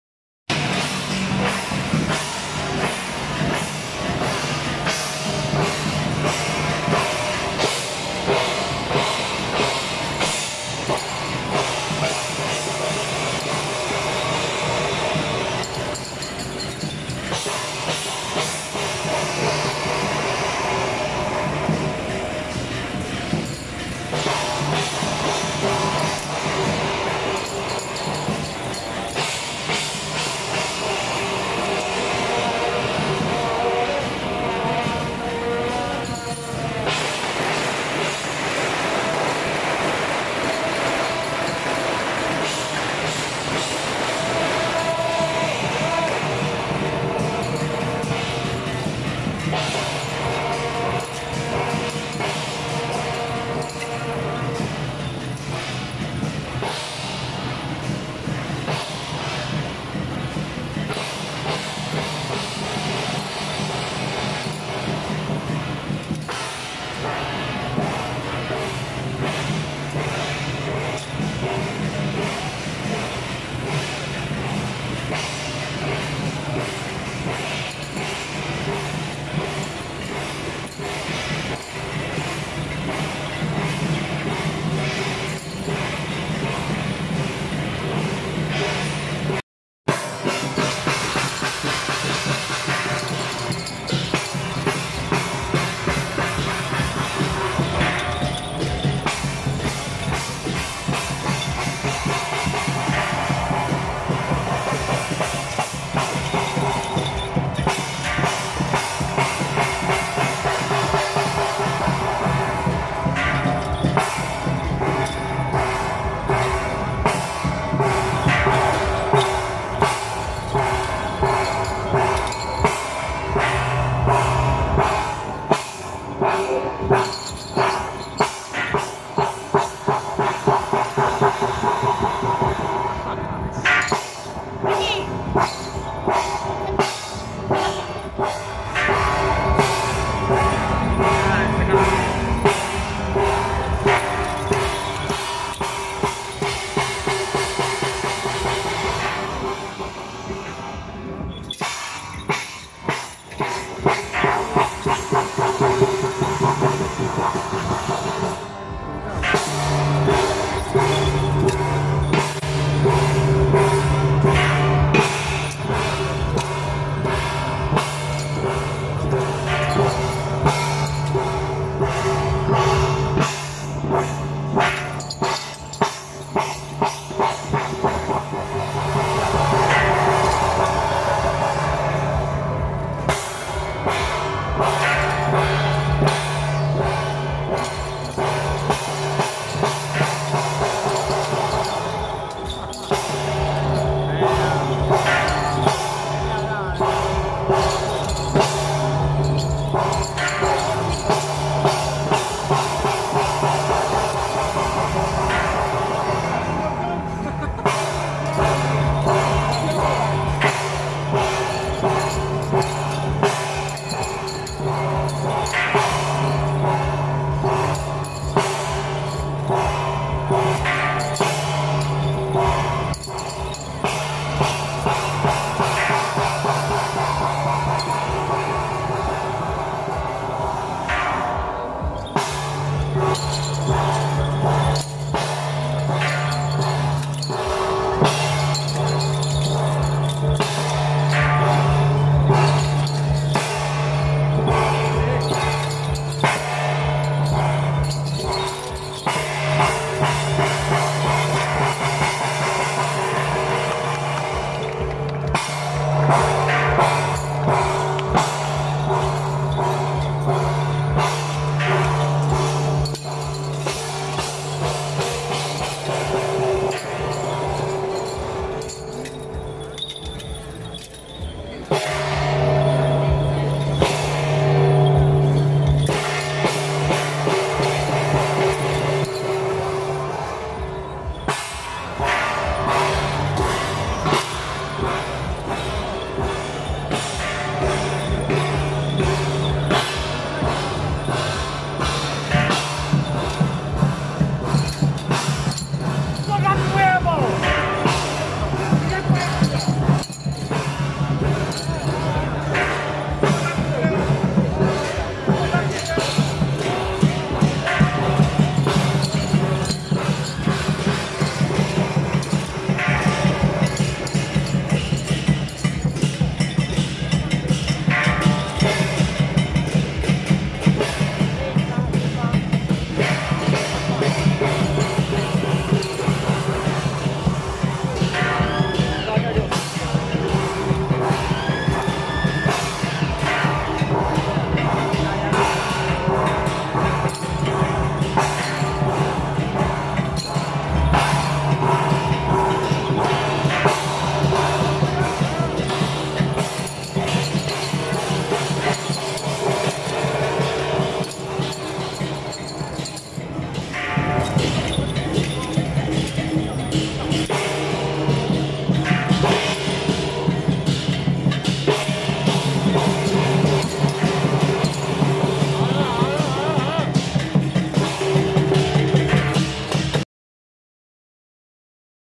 【汐止保王會-官將首】~109基隆碇內承福宮聚樂社西秦王爺,關聖帝君聖誕暨建館31週年遶境
汐止保王會-官將首